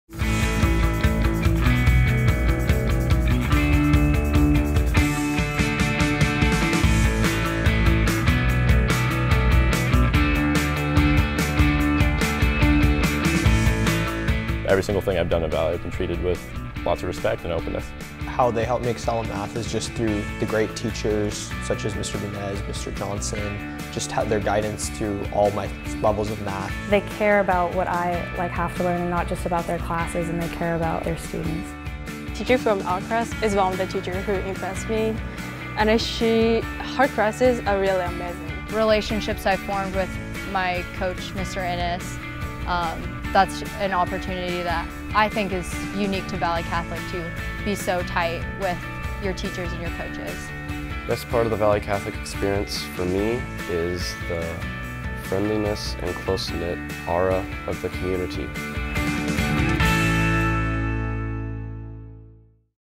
The Valley Catholic High School Experience: Student Features
Valley Catholic High School students share their stories about the variety of opportunities available to them from academics to activities to athletics.